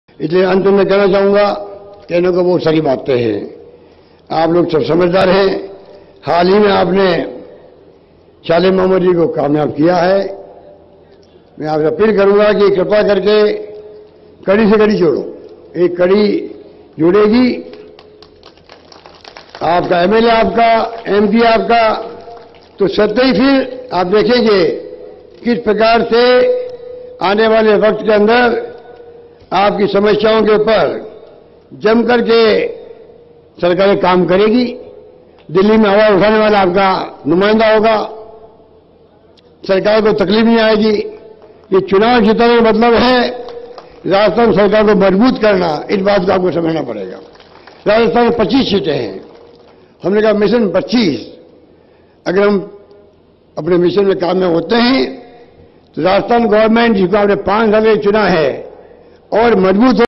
CM Ashok Gehlot will complete all the shortcomings in pokaran || india1newstv
CM अशोक गहलोत का पोकरण में सभी कमियों को पूरा करेंगे जल्दी